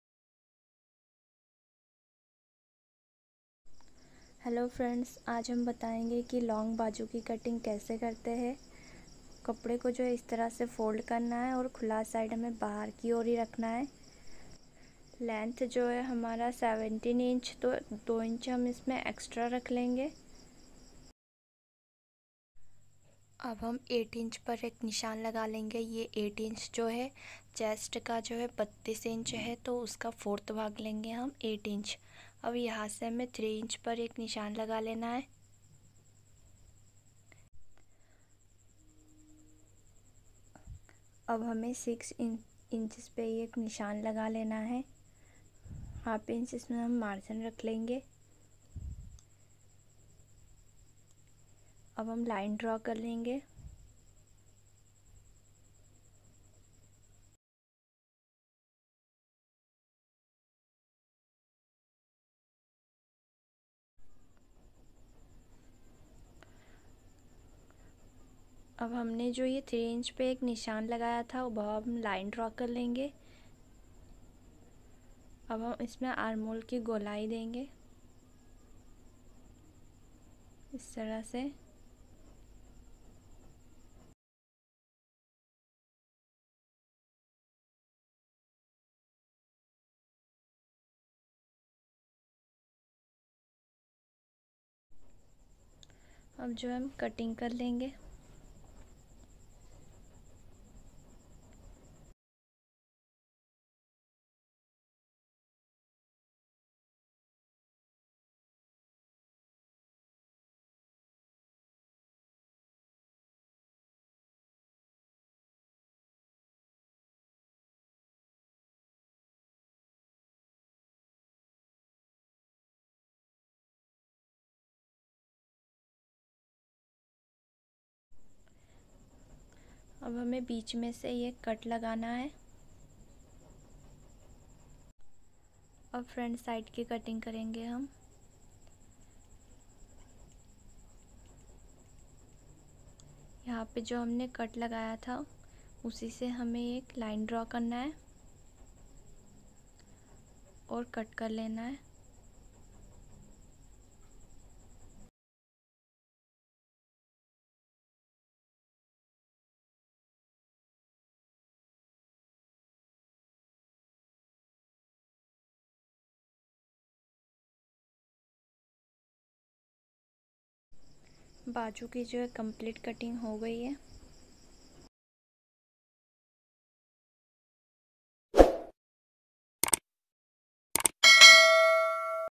Long Sleeves Cutting in Hindi . Baju Cutting Kaise Kare. Baju Cutting Very Easy Method.
Today I'll show you how to make Long Sleeves Cutting in Hindi . Baju Cutting Kaise Kare. Baju Cutting Very Easy Method..